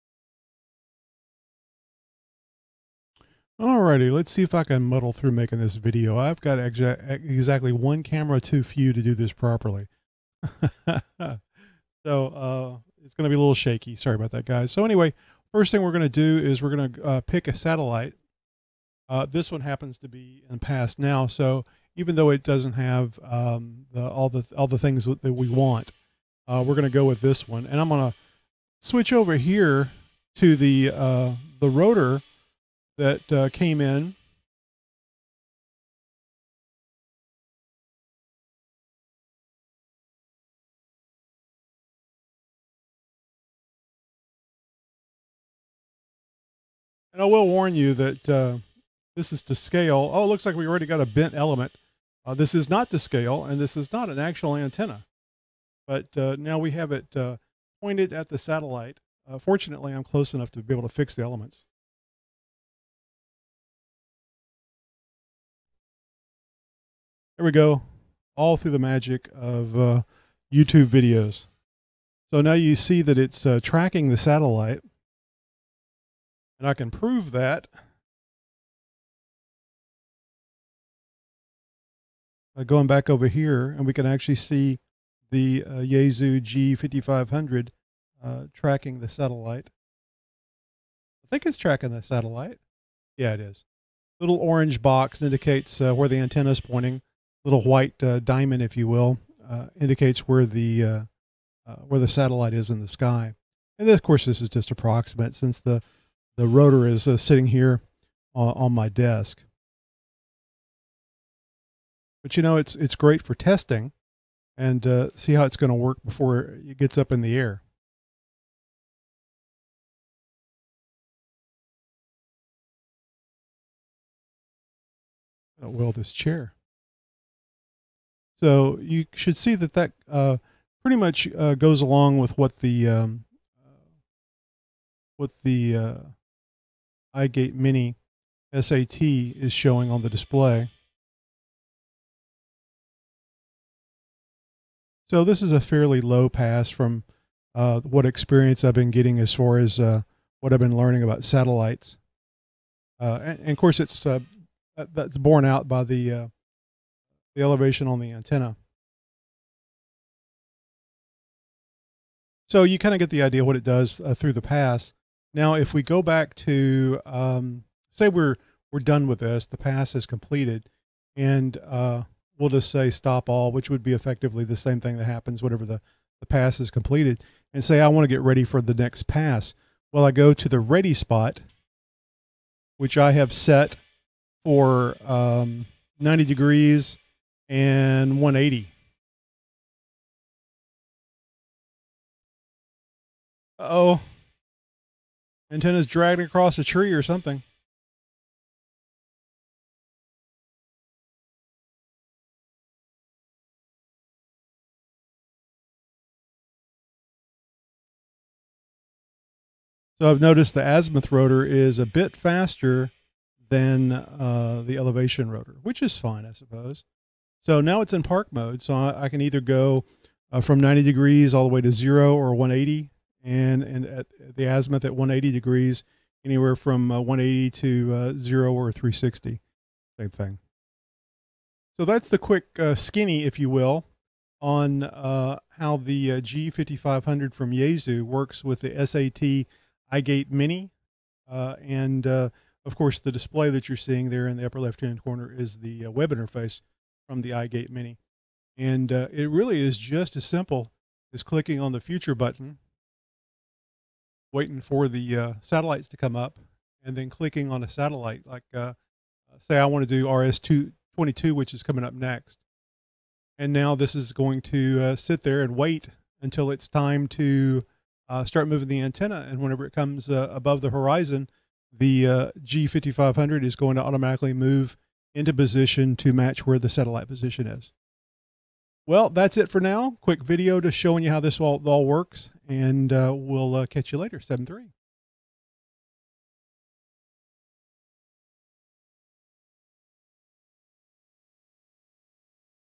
Satellite Ground Station Build - Yasue G-5500
The Yasue G-5500 arrived so I hooked it up to the iGateMini.com S.A.T. and ran this test!  This is going together very nice.  So much of the hard work is going to be done for me by computer, and that's what computers are supposed to do! :D  I purchased this Yasue G-5500 from HamRadio.com